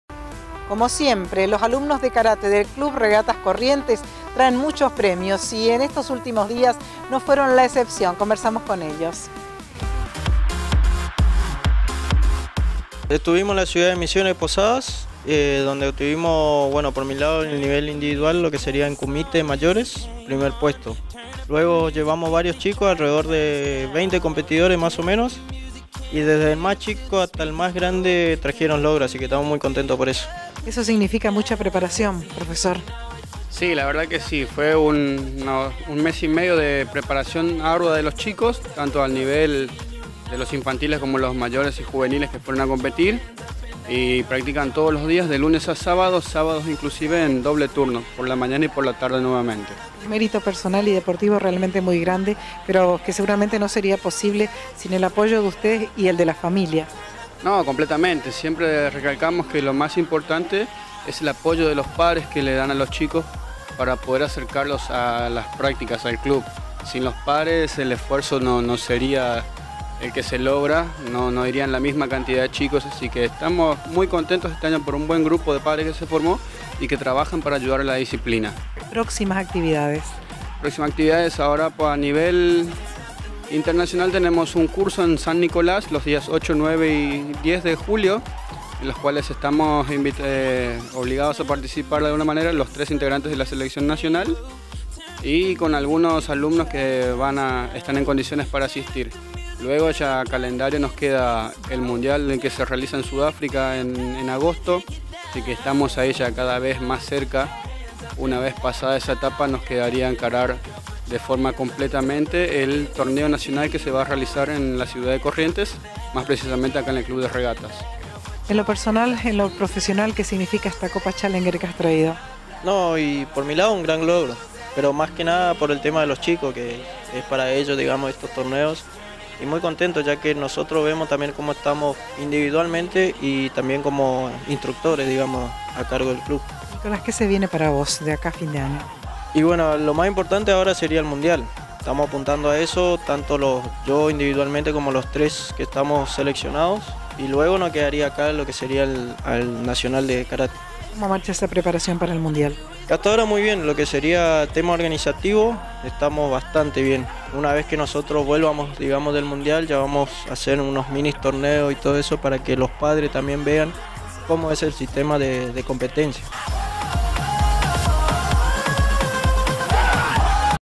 Club de Regatas Corrientes Karate
Al regreso de un Torneo dialogamos con los responsables del Karate del Regatas y vemos a los premiados . . .